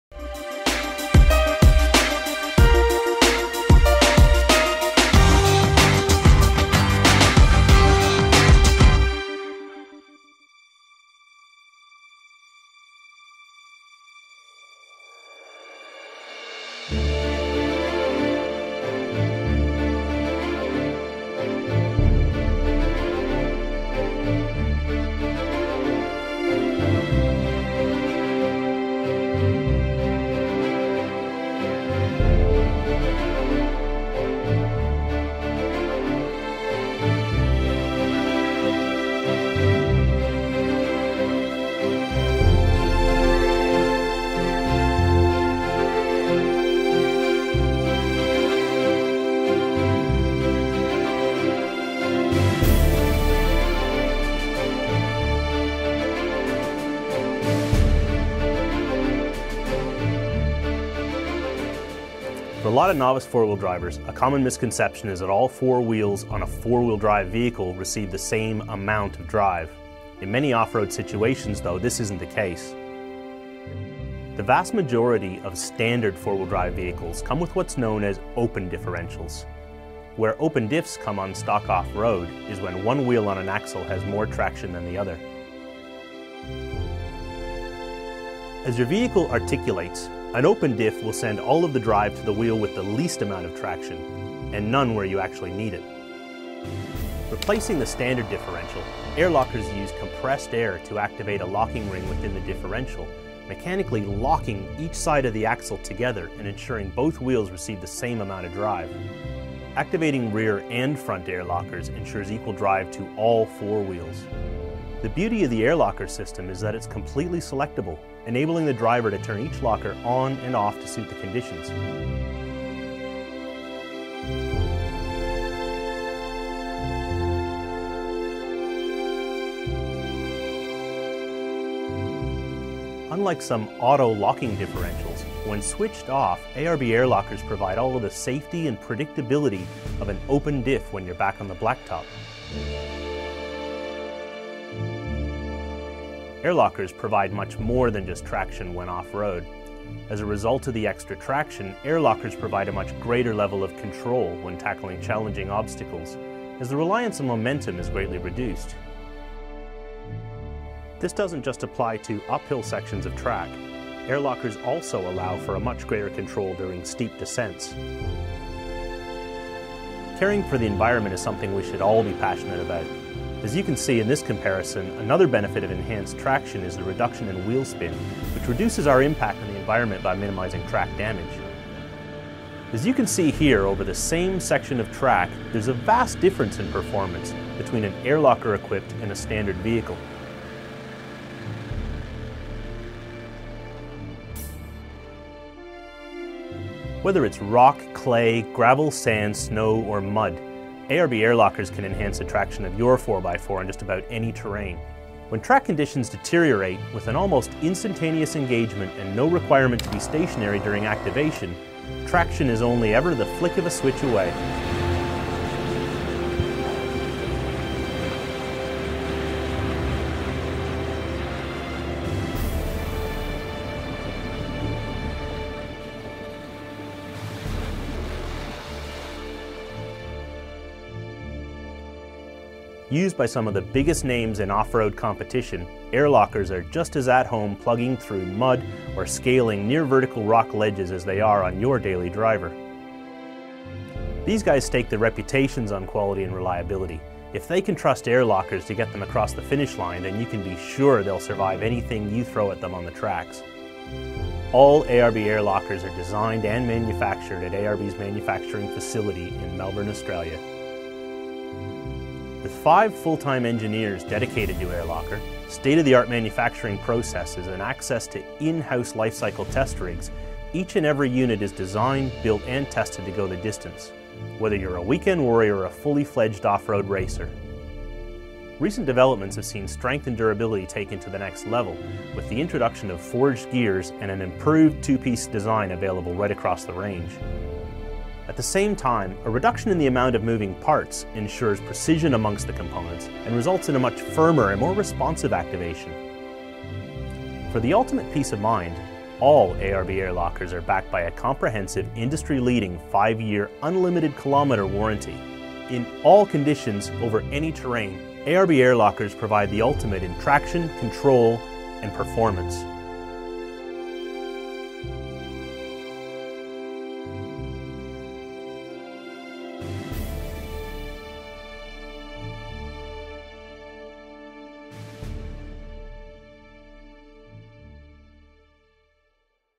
Blokada mechanizmu różnicowego ARB
Co to jest i jak działa blokada mechanizmu różnicowego na przykładzie blokady ARB